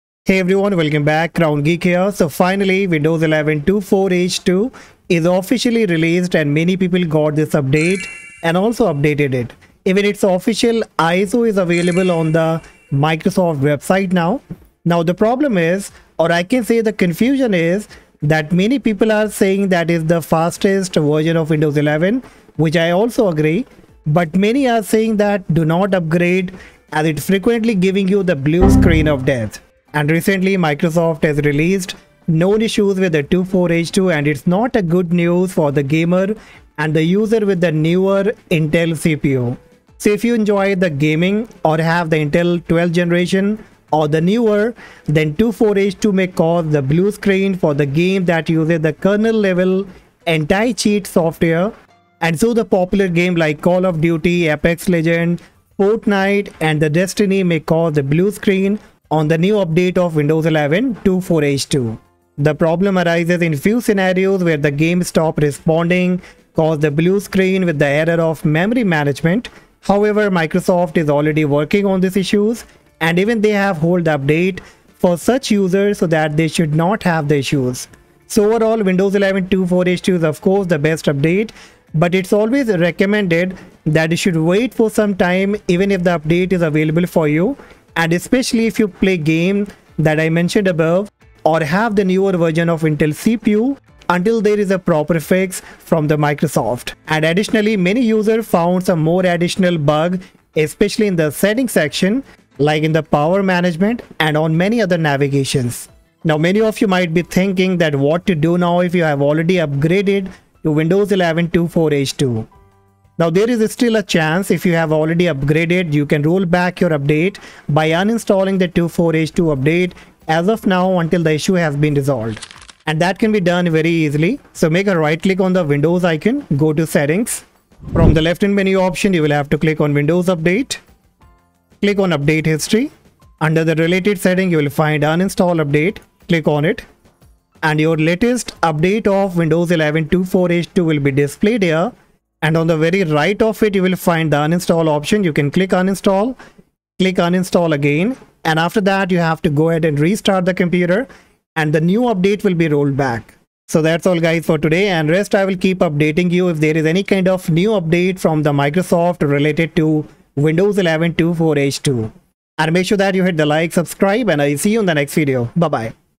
GAMERS & Intel New CPU Users - Don't UPGRADE to Windows 11 24H2
Recently Microsoft has released known issues with 24H2 and it's not good news for gamers and Users with New intel CPU
So if you enjoy gaming and have Intel 12th gen or newer 24H2, it may cause a blue screen for the games that use Kernel-level anti-cheat software. Such popular games like Call of Duty, Apex Legend, Fortnite, and Destiny may cause a Blue screen on a new update of Windows 11 24H2.

▬▬▬▬▬▬▬▬▬ஜ۩۞۩ஜ▬▬▬▬▬▬▬▬▬
▬▬▬▬▬▬▬▬▬ஜ۩۞۩ஜ▬▬▬▬▬▬▬▬▬

☑ Watched the video!
☐ Liked?